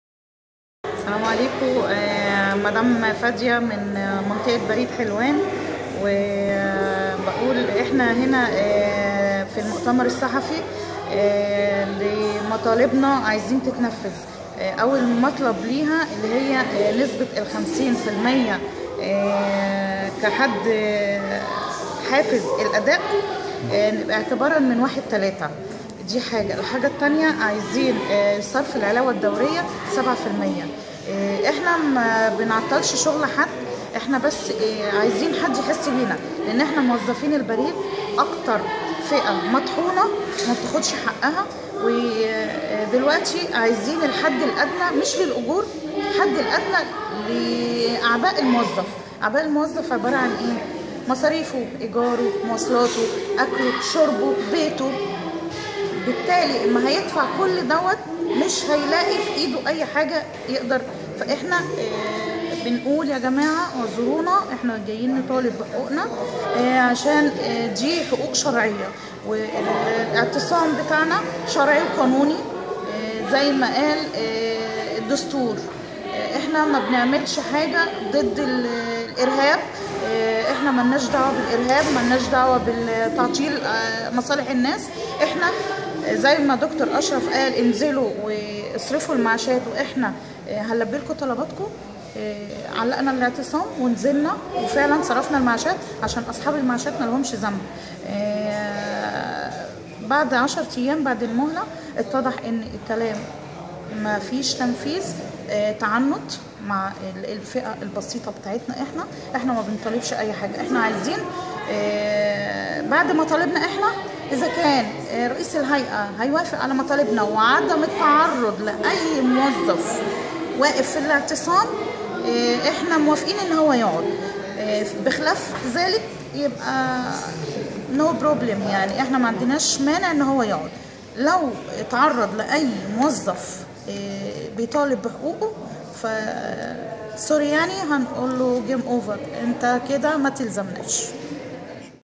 موظفة بهيئة البريد: نطالب بالحد الأدنى للمعيشة! | تلفزيون الاشتراكي
لقاء مع أحد الموظفات المشاركات في إضراب البريد، حول مطالب العاملين.